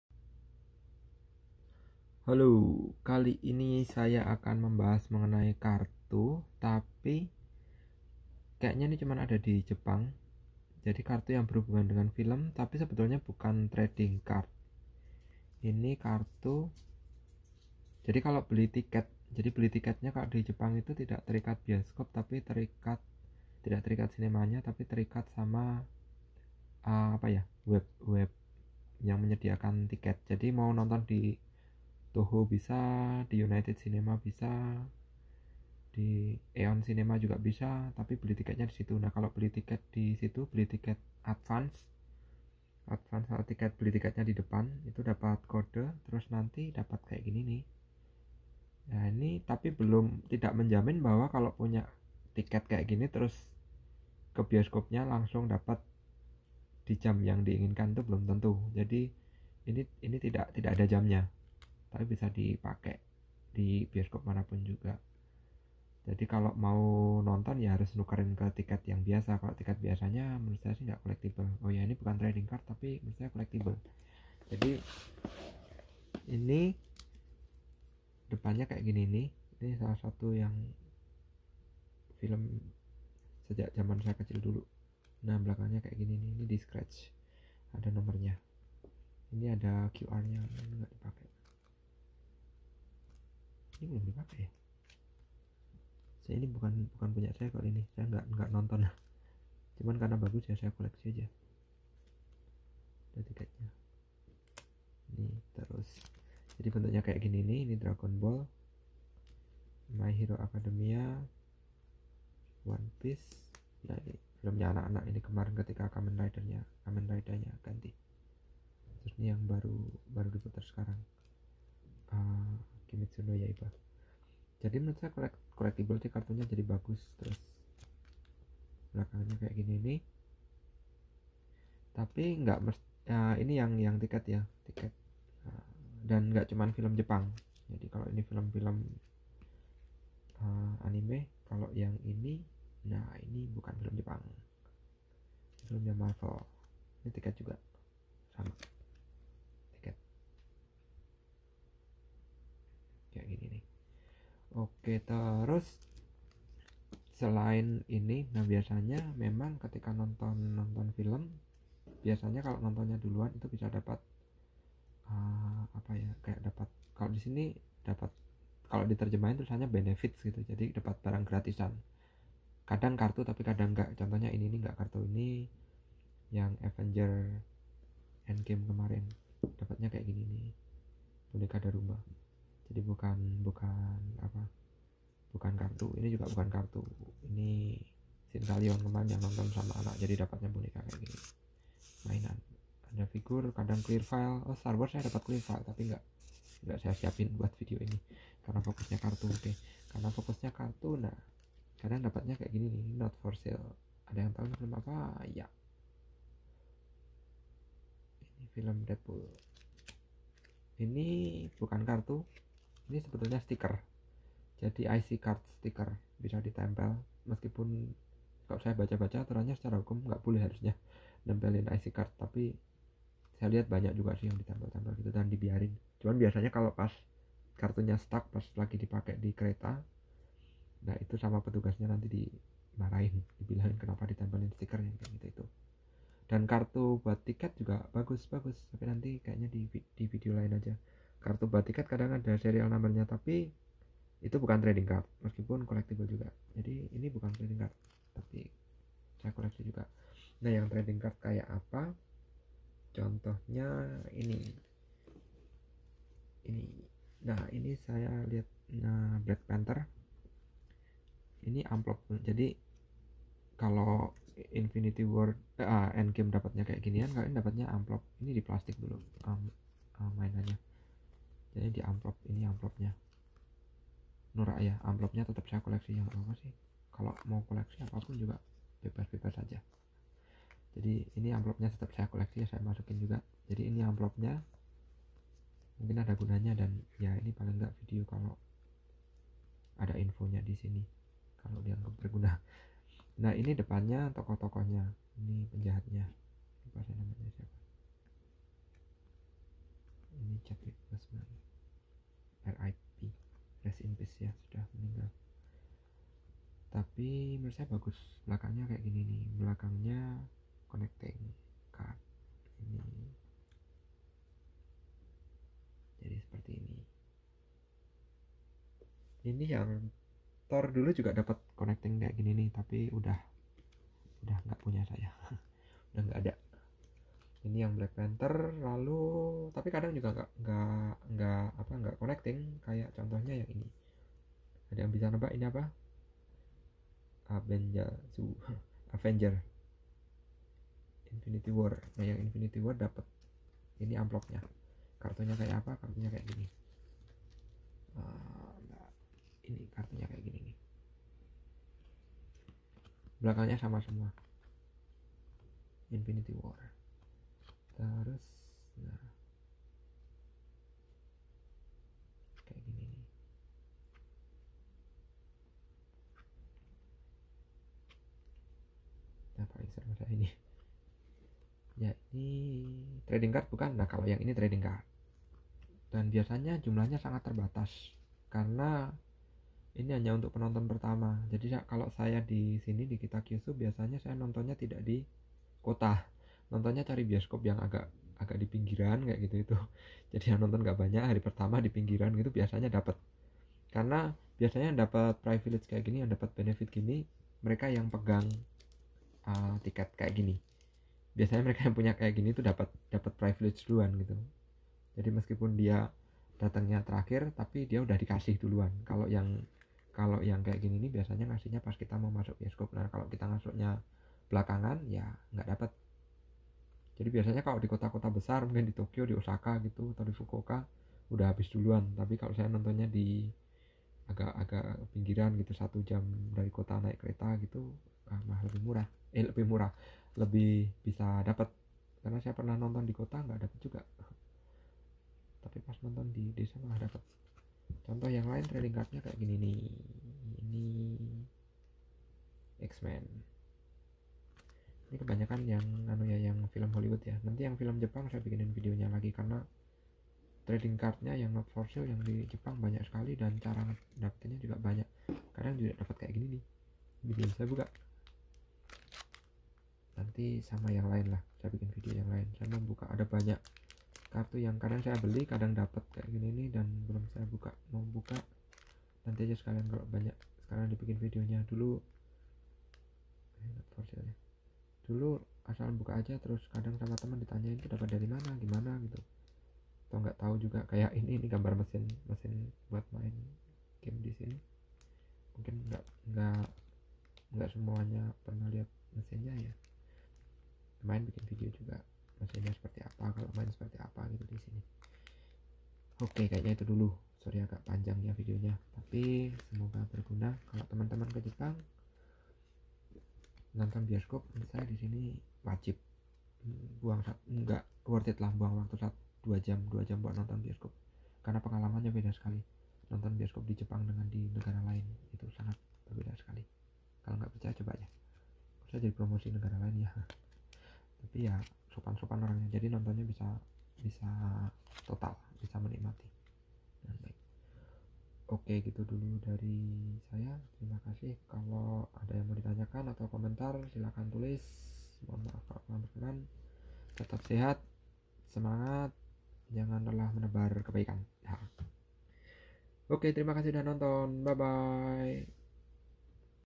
Movie related non-sports cards
Koleksi kartu terkait movie, atau anime? Gratis? Cara dapetin gimana?
Khas Jepang, ada kartu advanced ticket yang sangat collectible dan limited tapi bukan trading cards. Yang trading cards, ada juga. Not for sale, jelas limited juga dong-karena hanya dibagikan terbatas bagi penonton "pertama", dan sangat collectible.